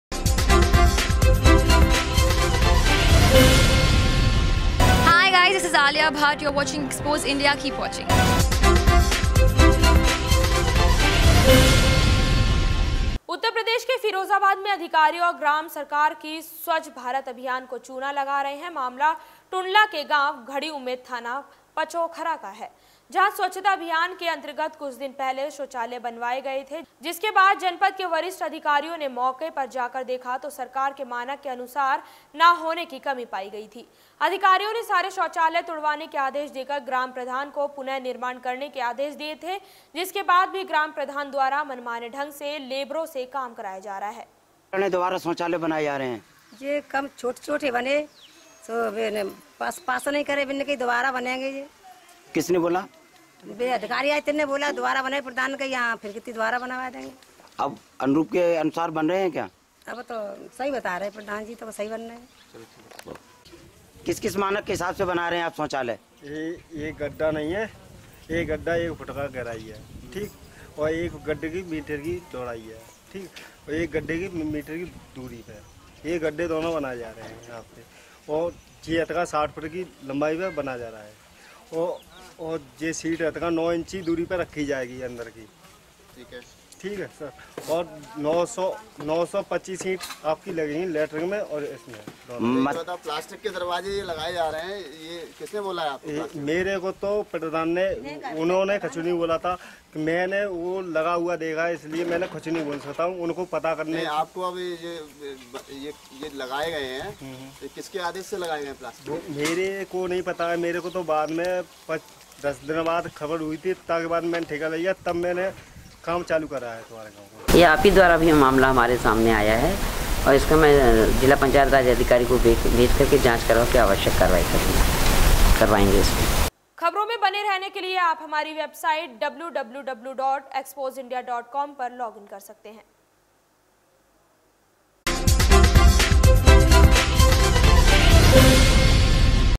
स्वच्छ भारत अभियान को अधिकारी लगा रहे पलीता
उत्तर प्रदेश के फिरोजाबाद में अधिकारी और प्रधान सरकार की स्वच्छ भारत अभियान को चूना लगा रहे हैं मामला टूण्डला के गांव घड़ी उम्मेद थाना पचोखरा का है। जहां स्वच्छता अभियान के अंतर्गत कुछ दिन पहले शौचालय बनवाए गए थे...जिसके बाद जनपद के वरिष्ठ अधिकारियों ने मौके पर जाकर देखा तो सरकार के मानक के अनुसार न होने की कमी पाई थी...अधिकारीयों ने सारे शौचालय तुड़वाने के आदेश देकर ग्राम प्रधान को पुनः निर्माण करने के आदेश दिए थे...जिसके बाद भी ग्राम प्रधान द्वारा मनमाने ढंग से लेबरों से काम कराया जा रहा है।